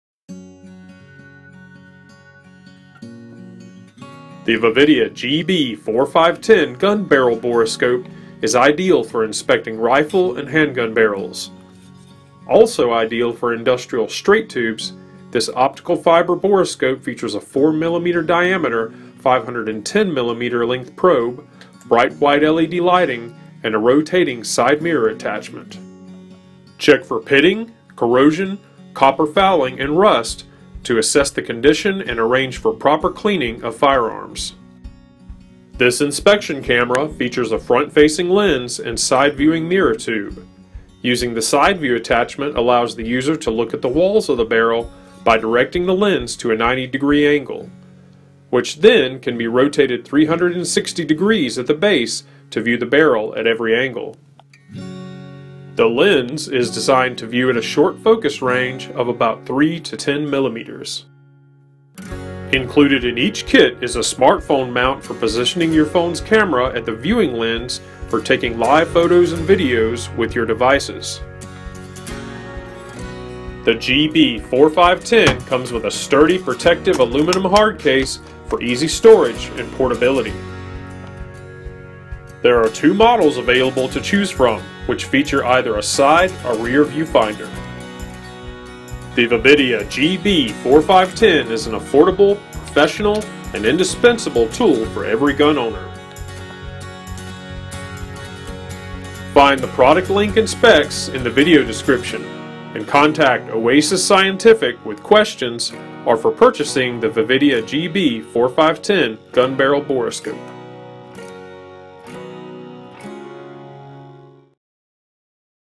Vividia GB-4510 Gun Barrel Borescope
Product Link/Amazon
The Vividia GB-4510 Rigid 4.0mm Borescope is designed specifically to inspect and examine gun barrels and other close focus range objects. There are two models with different viewer orientation. GB-4510R has viewer on rear-side of the borescope, while the GB4510S has side viewing observation window. The straight tube diameter is 4.0mm (0.16 inch) and length is 510mm (20 inch). The side-viewing mirror tube with diameter of 4.5mm (0.18 inch) can be fit nicely into the borescope tube to exam details of gun barrel walls. The mirror tube can be rotated 360 degrees freely. The phone attachment is used to view and record with IOS IPhones, and Android smartphones.
Specifications
Optical fiber pixels:                           7095
Diameter of inspection tube:          4.0mm (0.157inch)
Side View Mirror Diameter:             4.5mm (0.177inch)
Depth of View, Focus Distance:     5mm to 10mm
Water Resistant:                               Yes
Working Temperature:                     -20 C to +60 C
Lighting System:                               White light LED with PMMA light
Battery Required:                              One AA battery